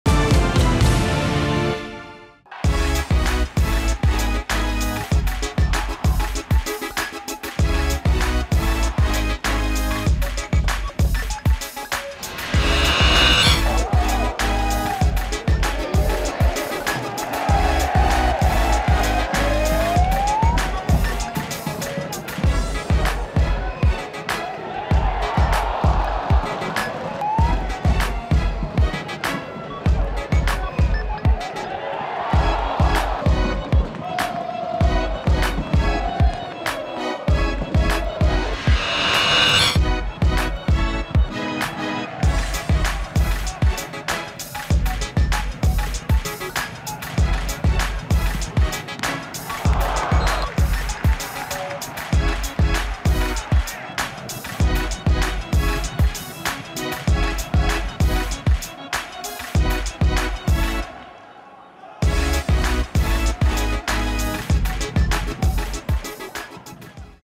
Top arrêts 2ème journée - Ligue 2 BKT / 2021-2022
Revivez les plus beaux arrêts de la 2ème journée de Ligue 2 BKT en vidéo.

Ligue 2 BKT - Saison 2021/2022 - 2ème journée

AC AJACCIO - AMIENS SC (3-1)
AJ AUXERRE - GRENOBLE FOOT 38 (0-0)
EN AVANT GUINGAMP - VALENCIENNES FC (1-1)
AS NANCY LORRAINE - TOULOUSE FC (0-4)
NÎMES OLYMPIQUE - DIJON FCO (2-1)
CHAMOIS NIORTAIS FC - STADE MALHERBE CAEN (0-1)
PARIS FC - USL DUNKERQUE (2-1)
QUEVILLY ROUEN METROPOLE - S.C. BASTIAIS (0-0)
RODEZ AVEYRON FOOTBALL - PAU FOOTBALL CLUB (1-0)
FC SOCHAUX-MONTBÉLIARD - HAVRE AC (0-2)